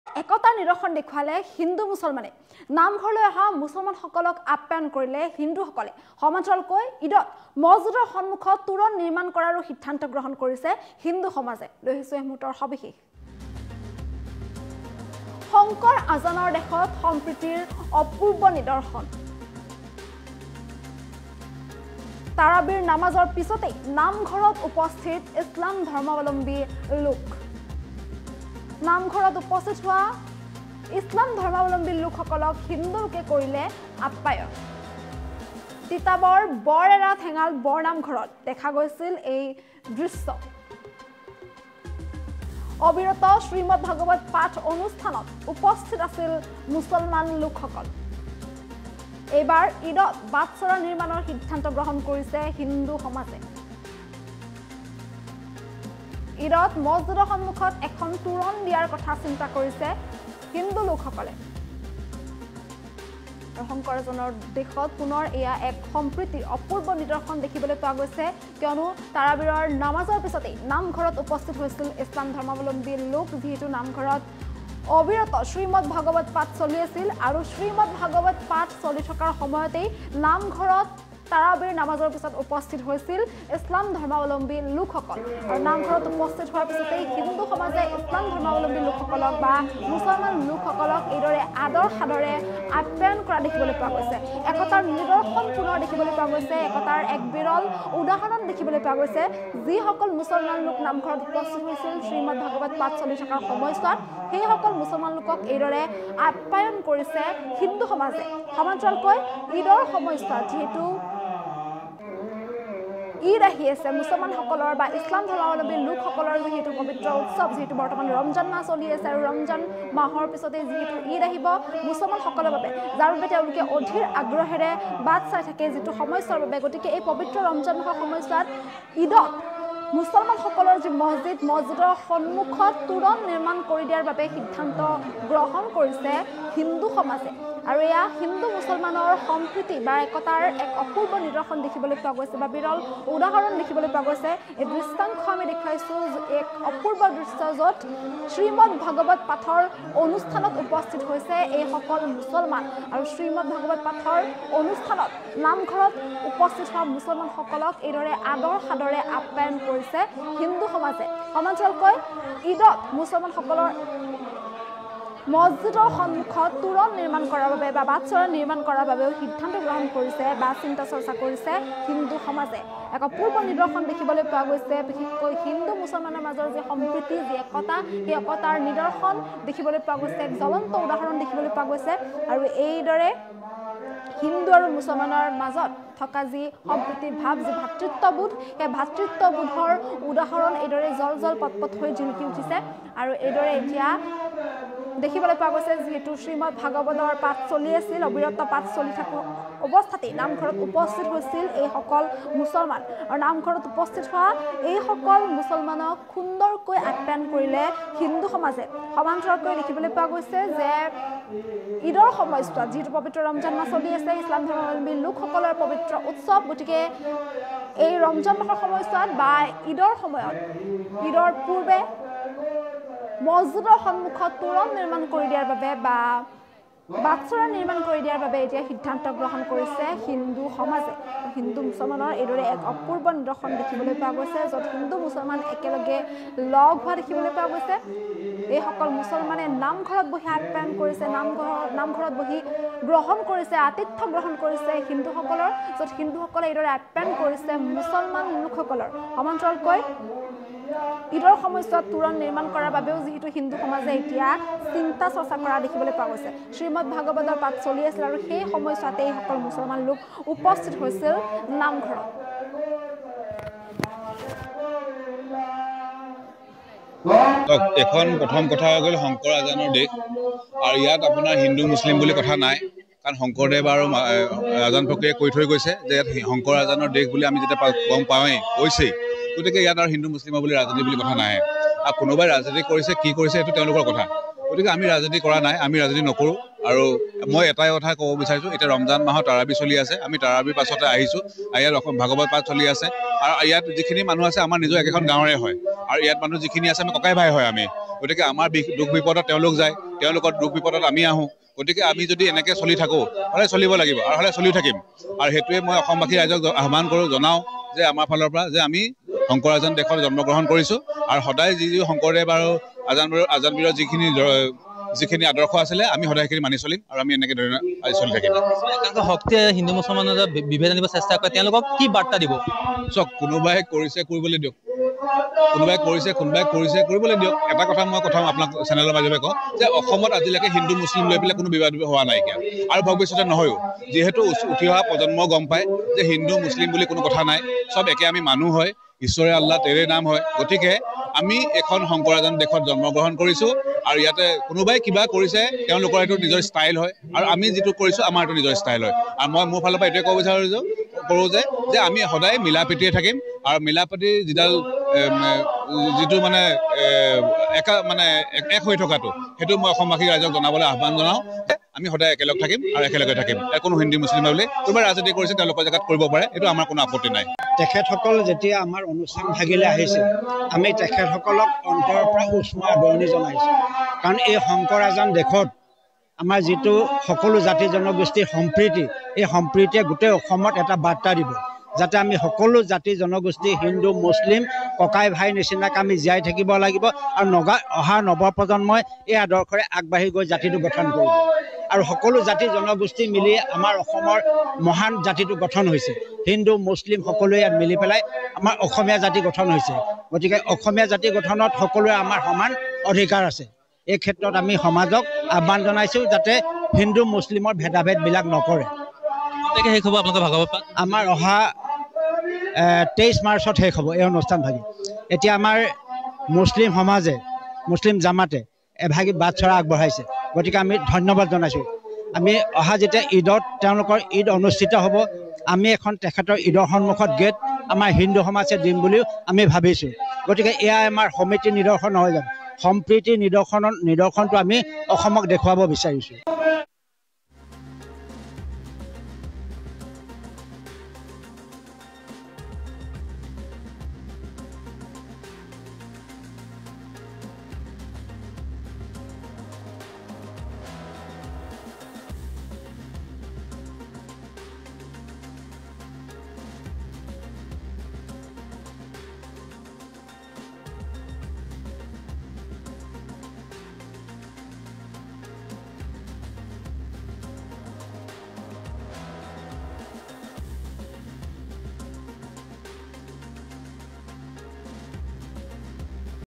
সম্প্ৰীতিৰ অপূৰ্ব নিদৰ্শন । পবিত্ৰ ৰোমজান মাহত ভাল লগা খবৰ ।
সম্প্ৰীতিৰ অপূৰ্ব নিদৰ্শন । পবিত্ৰ ৰোমজান মাহত ভাল লগা খবৰ । ভাগৱত পাঠ অনুষ্ঠানত অংশ লোৱা ইছলাম ধৰ্মীয় লোকক ঈদত হিন্দু সমাজে মছজিদৰ সন্মুখত নিৰ্মাণ কৰি দিব বাটচৰা ।